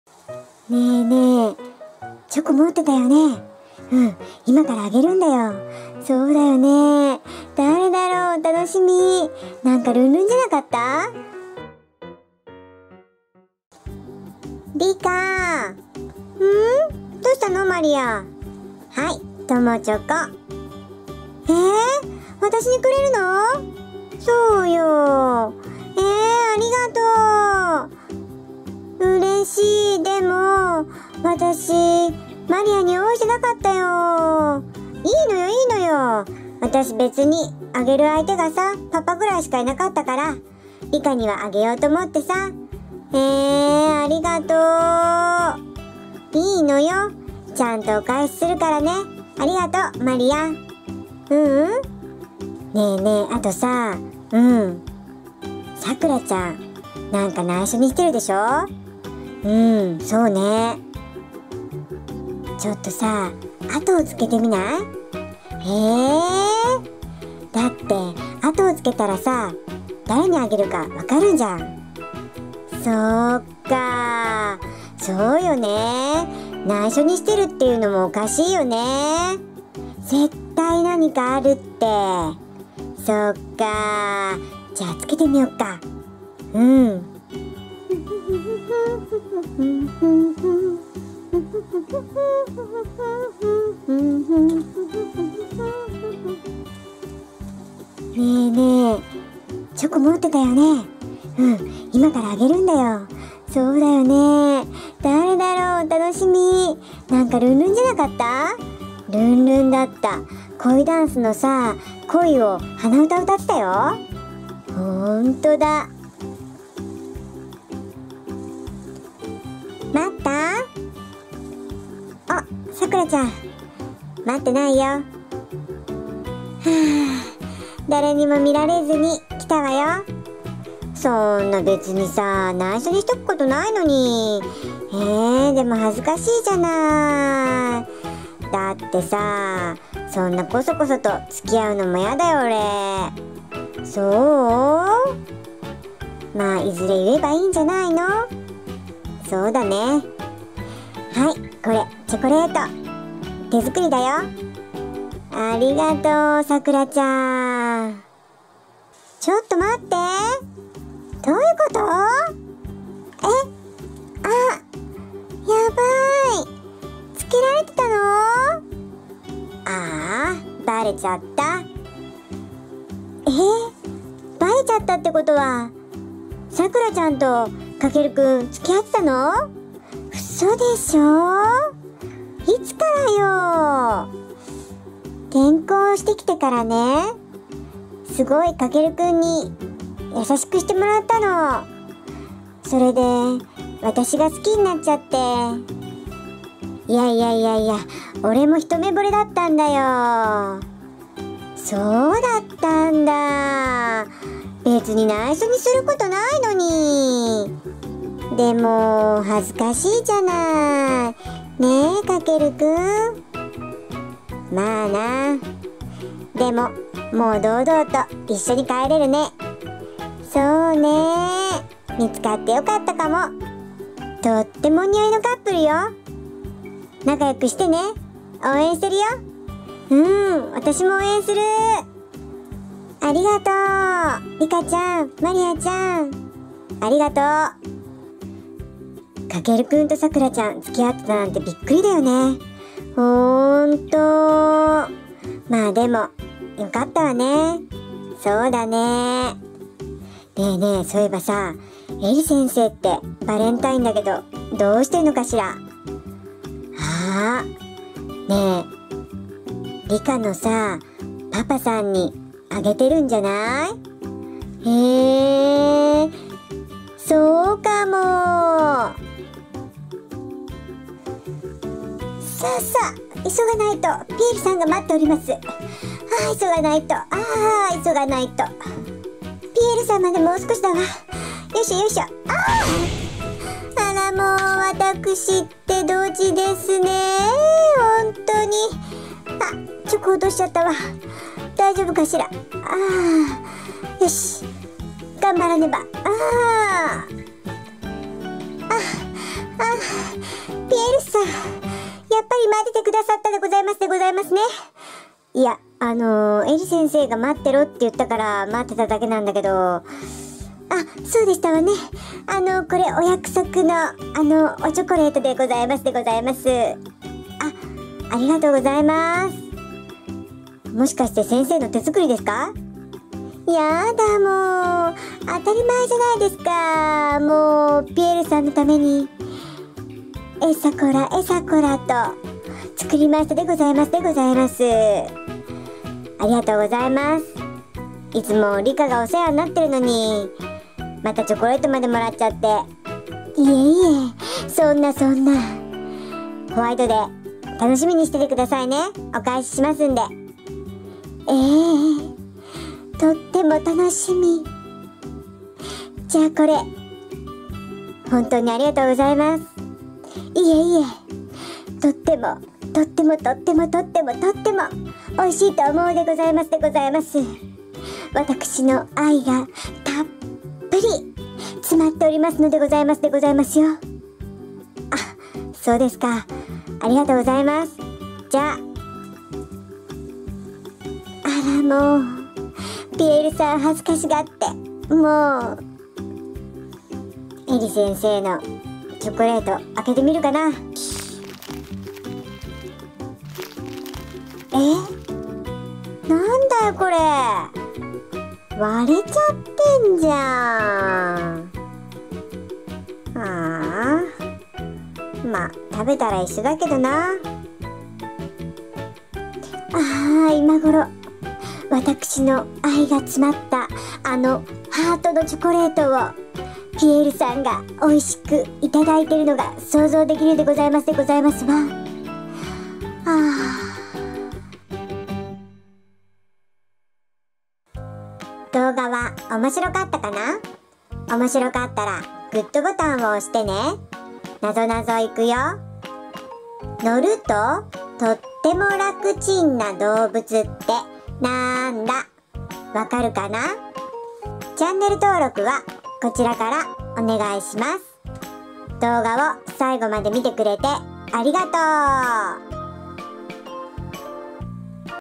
リカちゃん人形 アニメ☆バレンタイン♥さくらちゃん、エリ先生編☆おもちゃ ハウス おもしろ
今日はバレンタインデー♥
内緒にしていたさくらちゃんの相手を見る為に、リカちゃんとマリアちゃんはさくらちゃんのあとをつけると・・・！
一方、エリ先生はリカちゃんのパパにチョコを渡すが・・・！